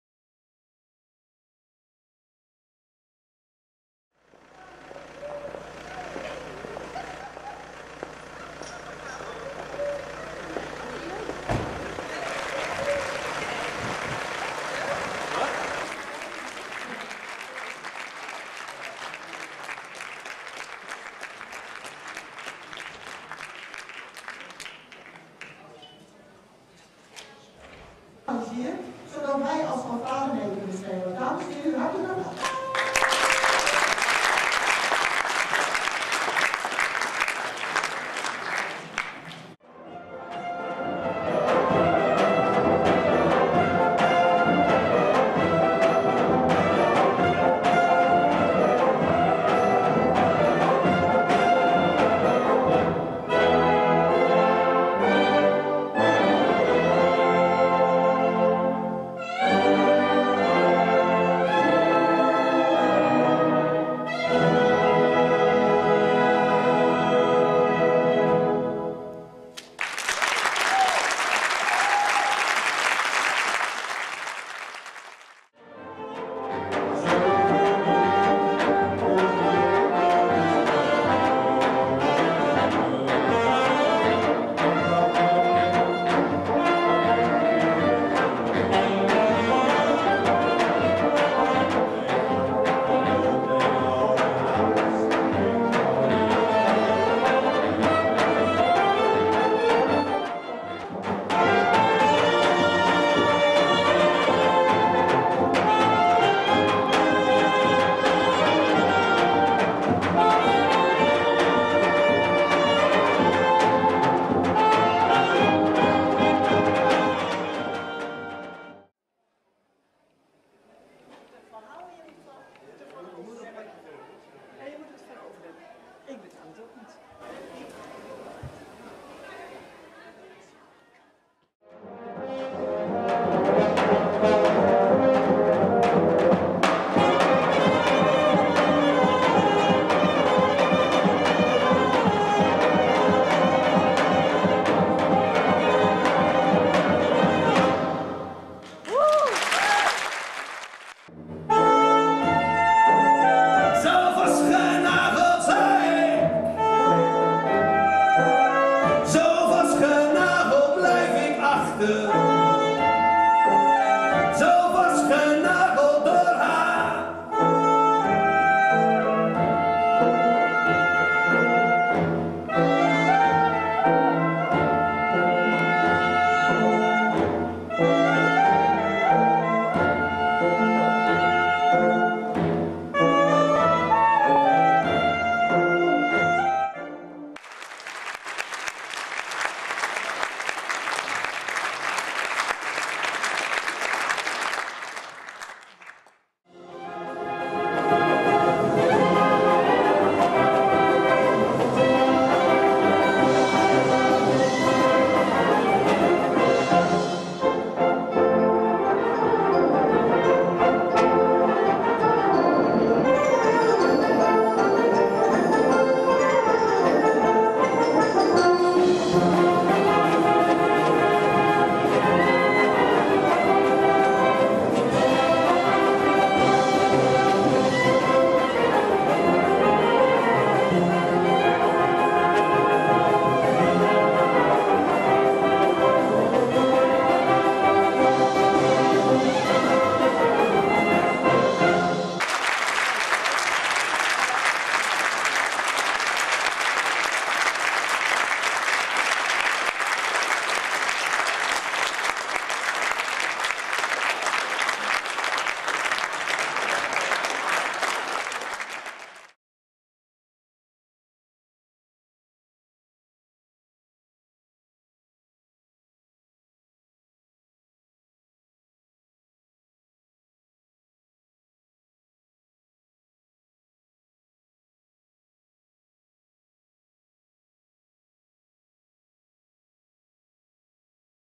wvds Kunst na Arbeid-1.mp4
Waarschuwing voor de Scheepvaart geeft een concert samen met Kunst na Arbeid in Purmerend. Balkanbrass met Fanfare.
WvdS bestaat uit Frans Limburg, Umit Catak, Frank Coornaert, Bart de Gans, Koen de Gans, Rein de Graaf, Henrik Holm, KNA staat onder leiding van Lieuwe de Jong.